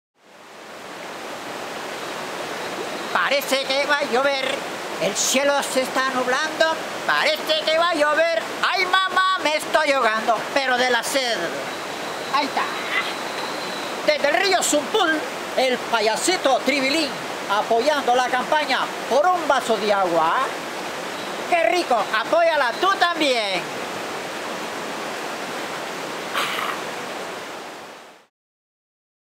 POR UN VASO DE AGUA DESDE CHALATENANGO, EL SALVADOR, C.A.
El payasito Triviling se une a la campaña "POR UN VASO DE AGUA" Desde el majestuoso río Sumpul en Chalatenango, El Salvador, C.A.